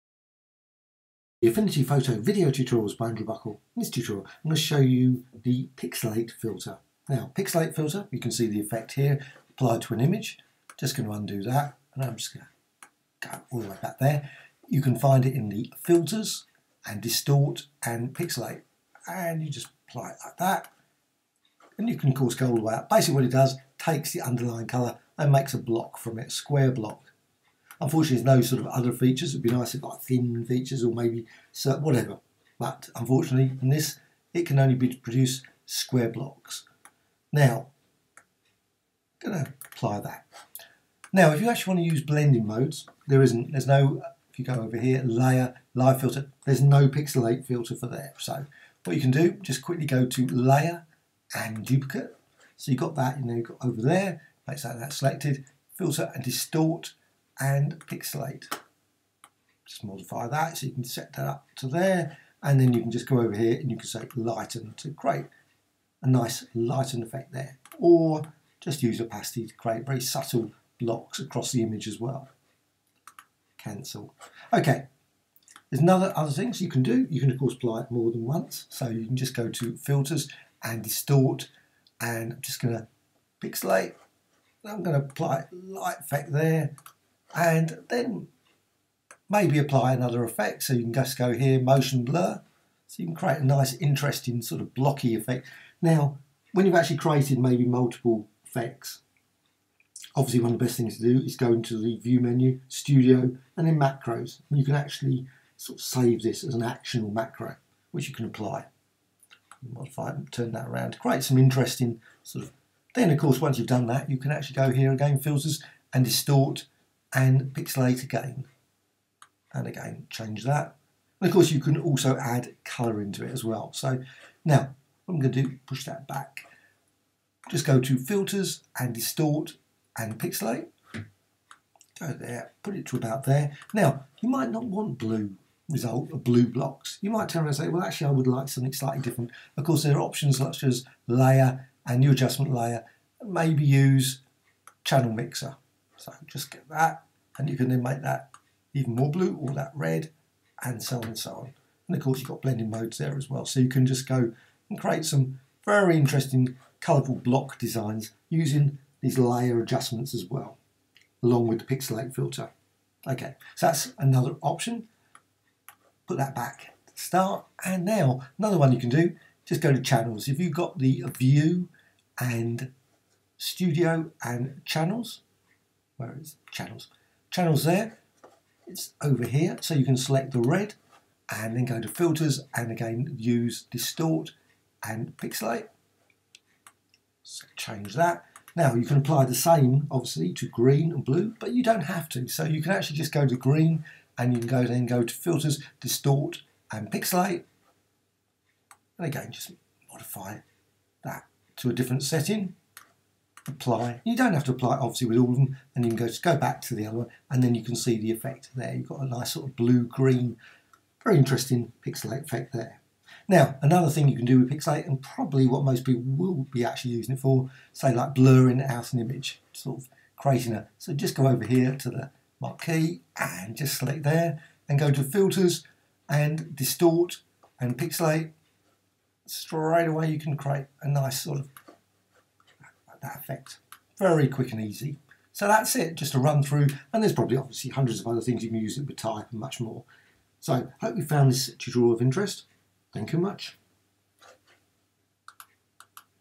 Pixelate Filter In Affinity Photo tTutorial | How To | Quantise | Square Blocks | Graphicxtras
Pixelate filter in Affinity Photo tutorial / guide - how to use the pixelate filter to create all kinds of wonderful effects applied to layers and images etc. The pixelate filter can be used in many different ways in Affinity Photo